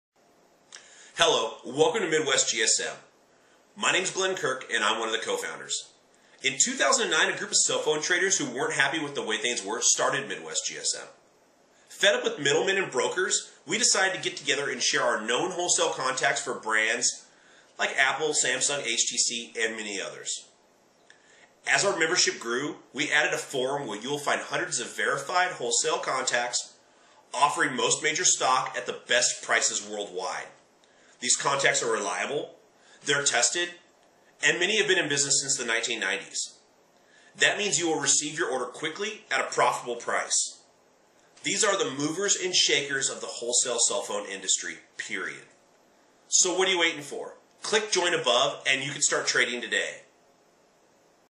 What is MidwestGSM?
In 2009, a group of cell phone traders who weren't happy with the way things were started MidwestGSM. Fed up with middlemen and brokers they decided to get together and share known wholesale contacts for brands like Apple and Samsung. As their membership grew they added a forum where you will find hundreds of verified wholesale contacts offering most major stock at the best prices worldwide. JOIN NOW and you can start trading today.